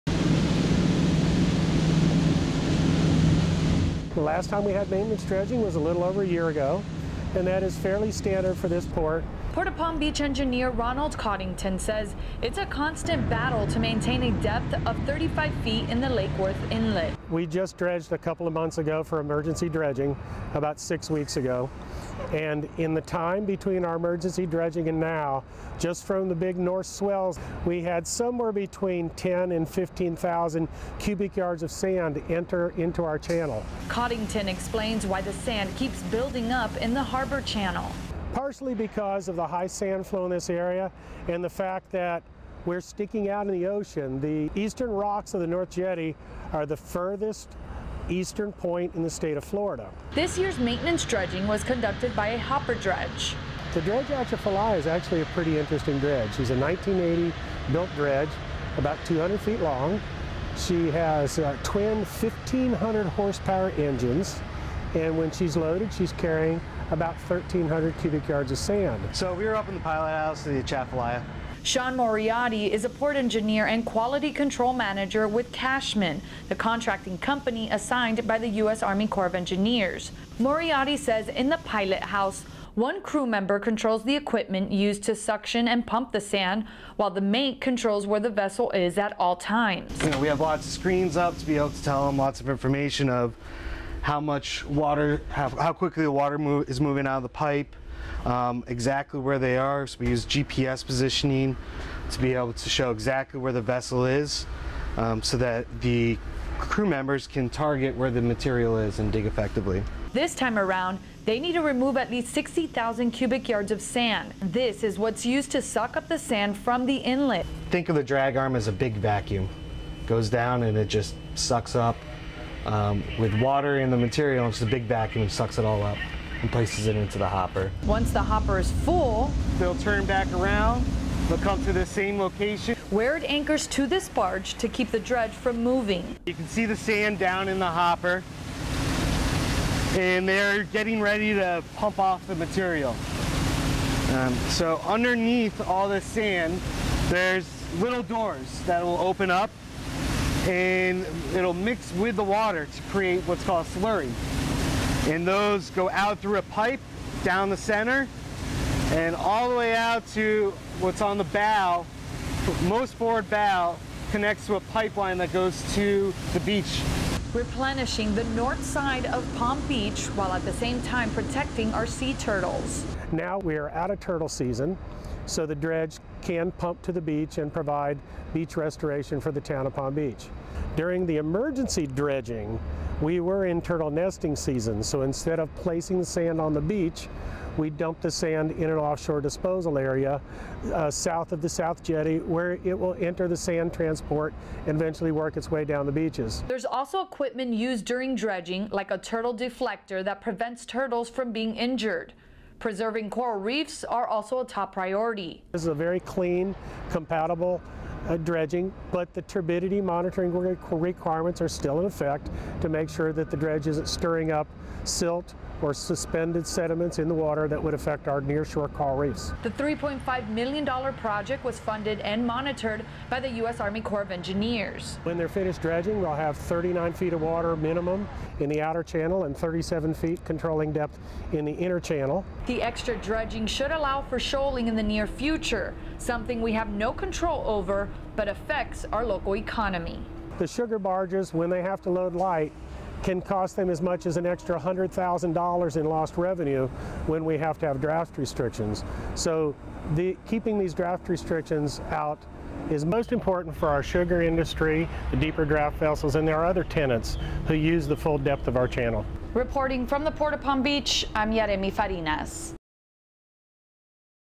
Inside the dredging of the Lake Worth Inlet
A look inside the 2019 maintenance dredging of the Lake Worth Inlet